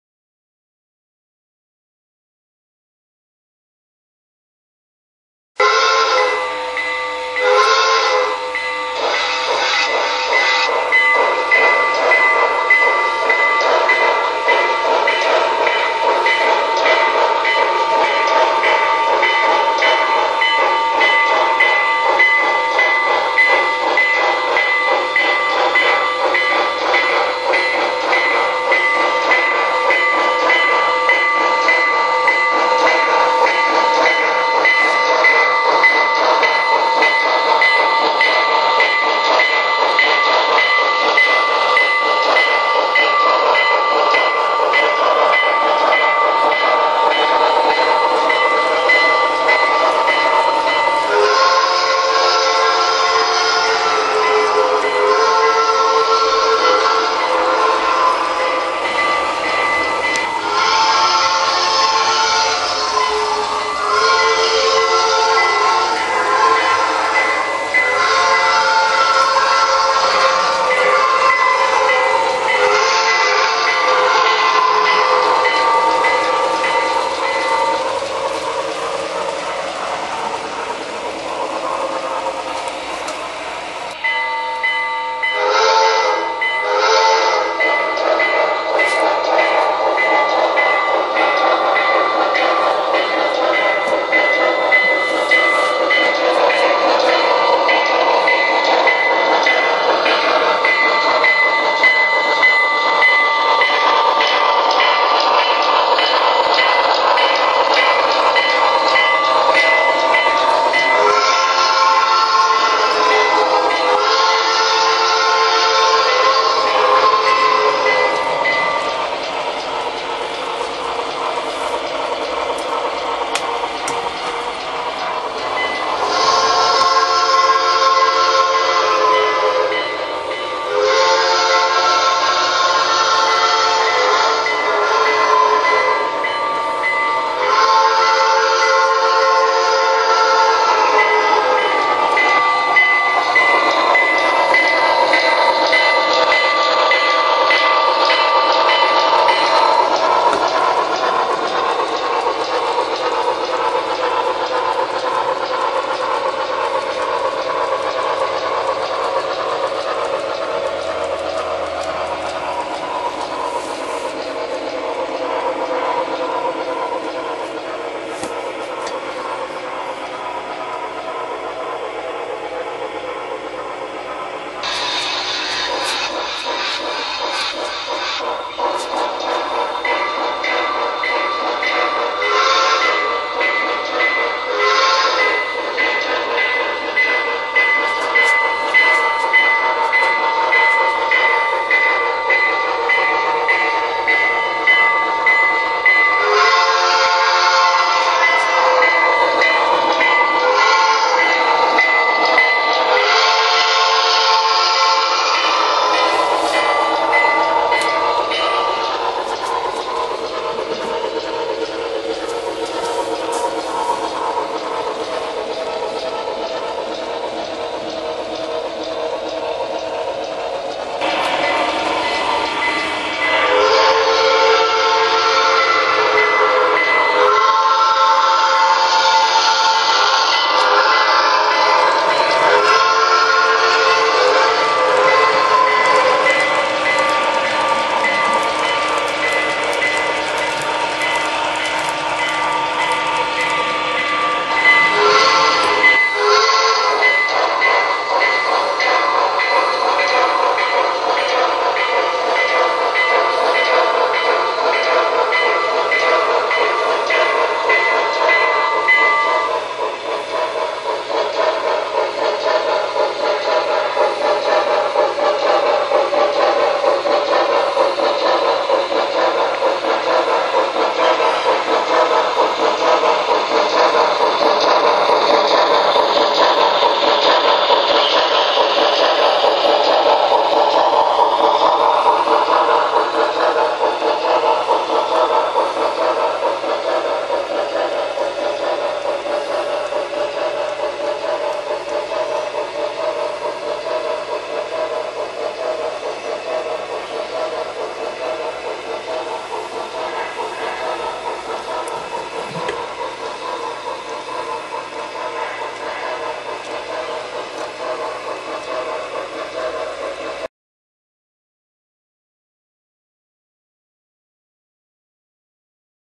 Pennsy 2-10-0 Hippo #4620 Action!
Video of Pennsy 2-10-0 #4620 ( a Broadway Imports) on my layout. Filmed by Jim Gray 2/28/12.